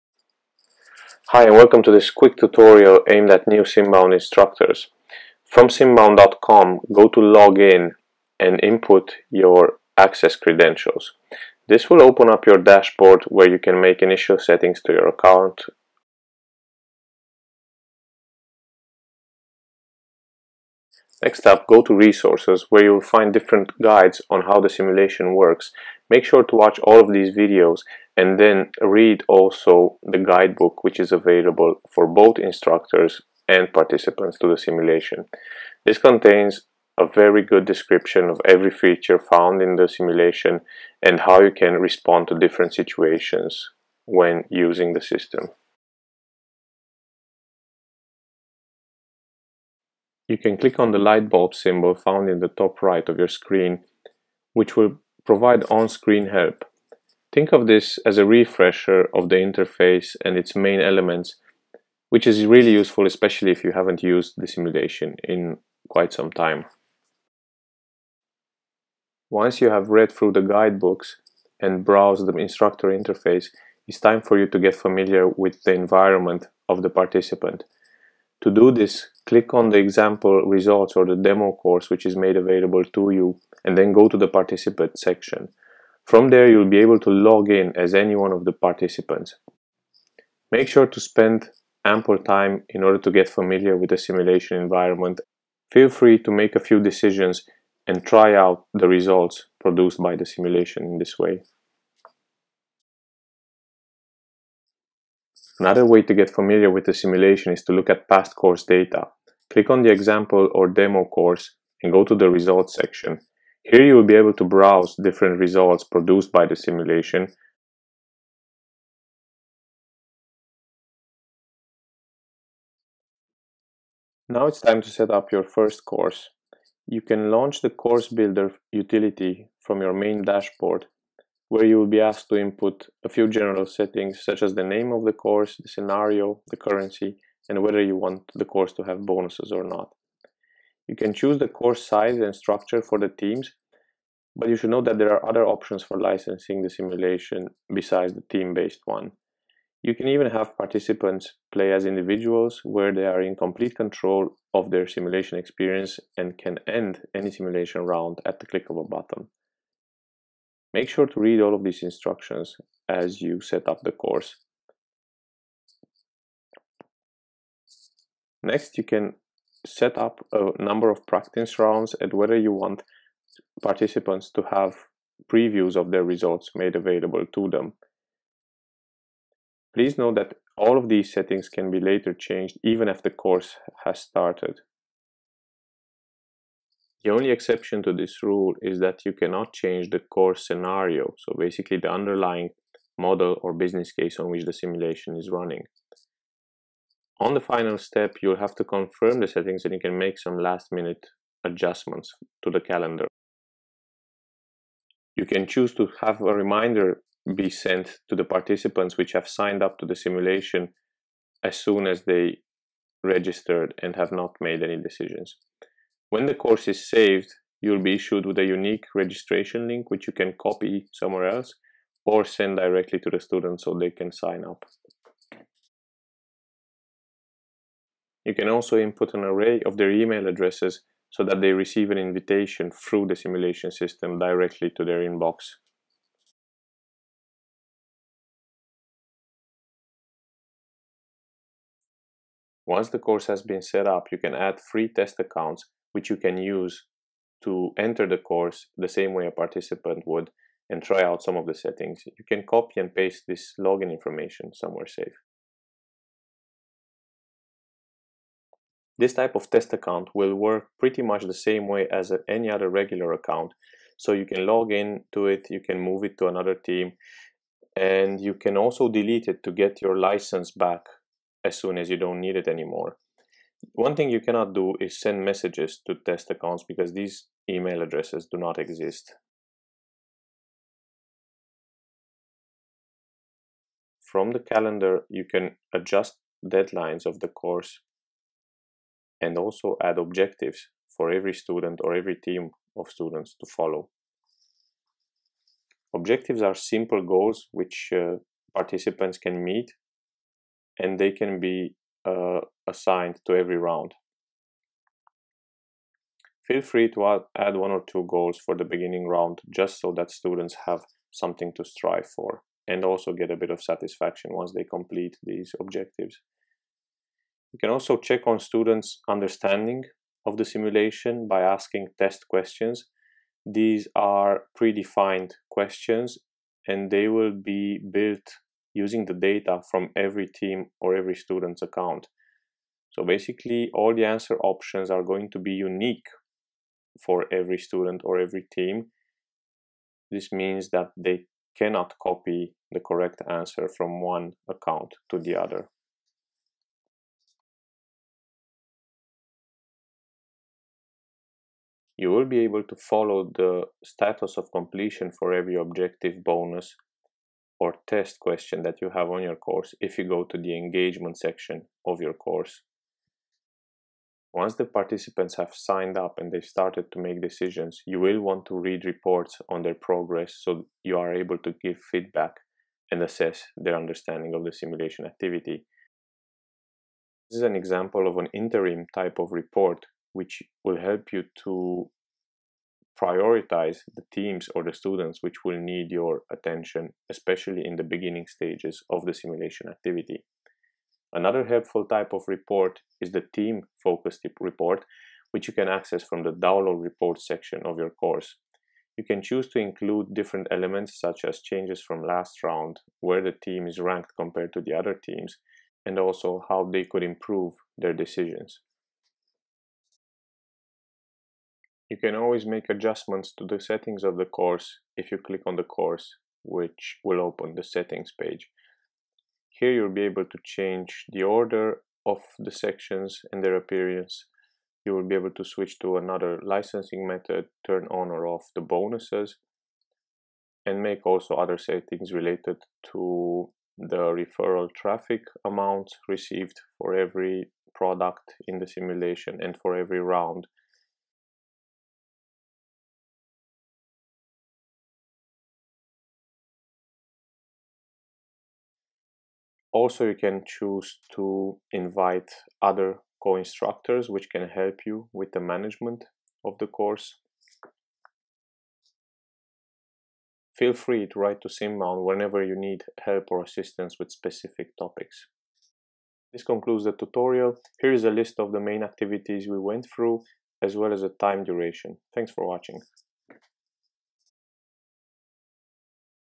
New to teaching with Simbound? Set up a simulation-based course in minutes!
Brief introduction of Simbound aimed at instructors & tutors. Video will show you how to set up your account, learn about the simulation environment, where to find guides and on-screen help.

You will be shown how to create a course step-by-step and how to consult various reports and resources that you can use to provide feedback and learning to the participants of your course.

You will need to hold a valid instructor account in order to perform all of the operations presented on here. To access the simulation and other teaching resources